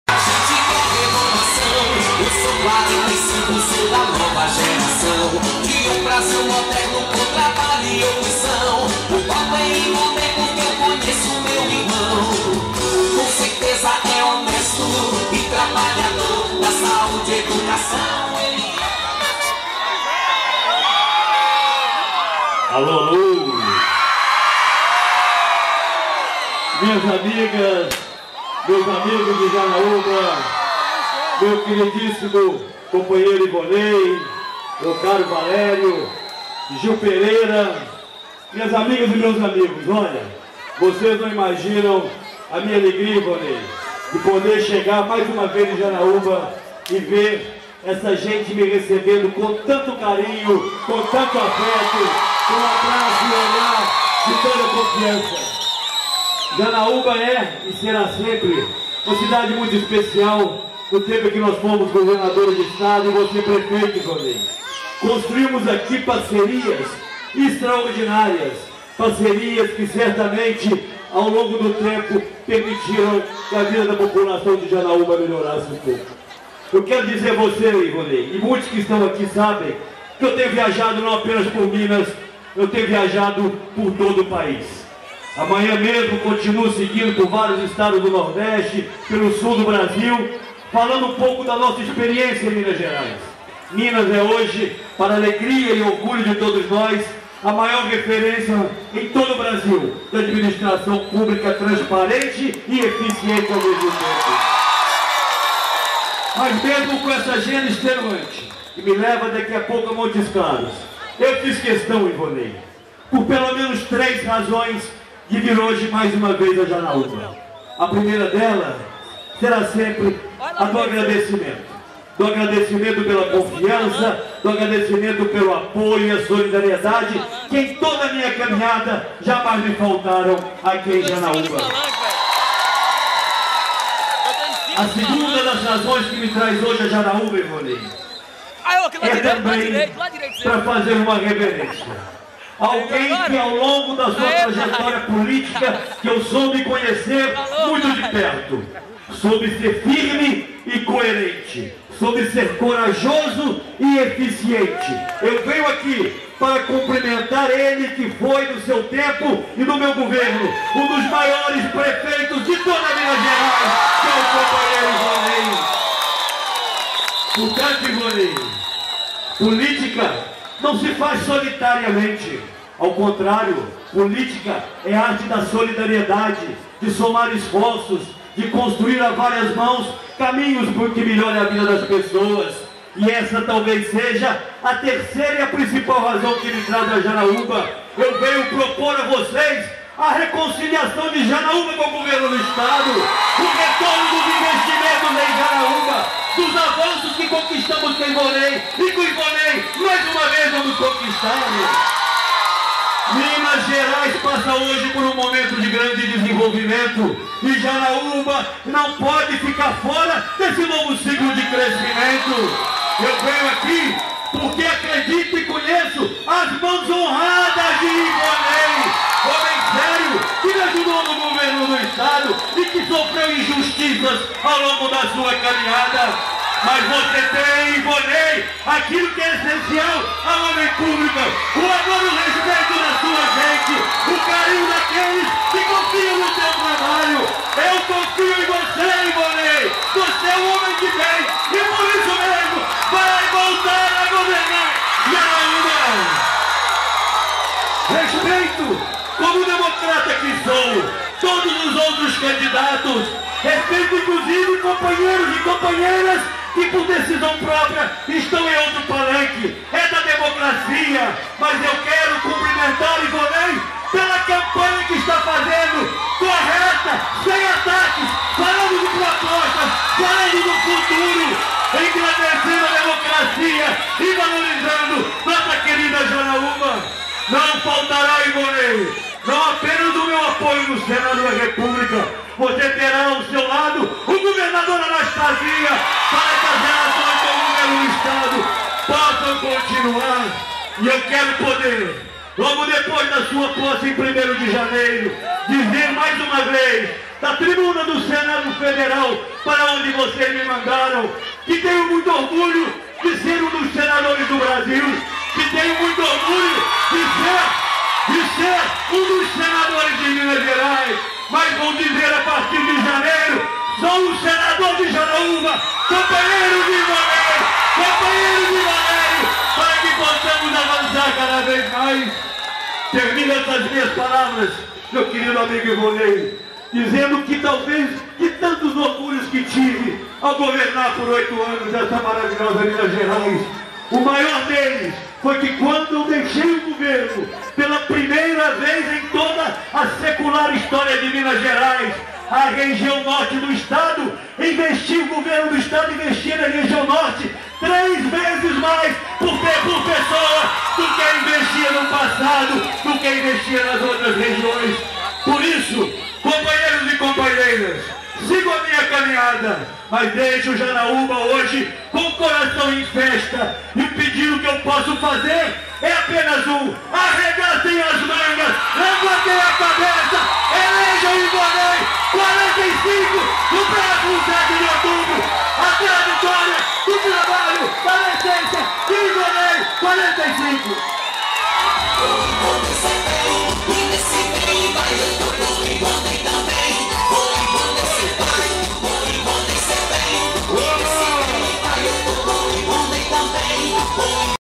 AÉCIO NEVES COM IVONEI, VALÉRIO DIAS E O POVO EM JANAÚBA 2012
Comício de Ivonei Abade e Valério Dias com a presença do senador Aécio Neves na avenida do Comércio, em Janaúba, no dia 21 de setembro de 2012.